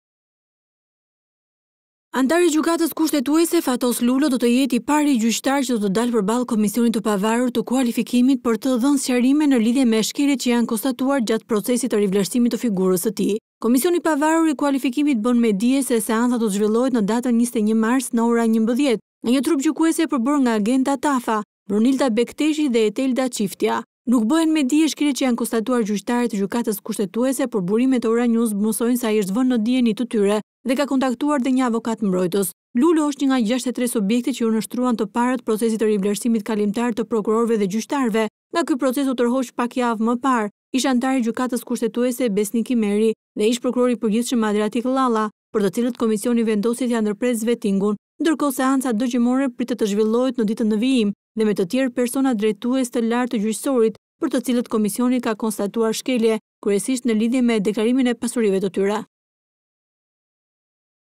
Ora News - Thirret i pari, KPK kërkon anëtarin e Gjykatës Kushtetuese Fatos Lulo
Anëtari i Gjykatës Kushtetuese Fatos Lulo do të jetë i pari gjyqtar që do të dalë përballë Komisionit të Pavaruar të Kualifikimit për të dhënë sqarime ne lidhje me shkeljet që i janë konstatuar gjatë procesit të rivlerësimit të figurës së tij.

Komisioni i Pavaruar i Kualifikimit bën me dije se seanca do të zhvillohet në datën 21 mars në ora 11:00 nga një trup gjykues i përbërë nga Genta Tafa, Brunilda Bekteshi, Etleda Çiftja.

Nuk bëhen me dije shkeljet që i janë konstuar gjyqtarit të Gjykatës Kushtetuese, por burime të Ora Neës mësojne se ai është vënë në dijeni të procesit dhe për to ka kontaktuar edhe një avokat mbrojtës.

Lulo është një nga 63 subjektet që iu nënshtruan të parët procesit të rivlerësimit kalimtar të prokurorëve dhe gjyqtarëve.

Nga ky proces u tërhoqën pak javë me parë ish- anëtari i Gjykatës Kushtetuese Besnik Imeri dhe ish -prokurori i Përgjithshëm Adriatik Llalla, për të cilët Komisioni vendosi t'ua ndërpresë vettingun.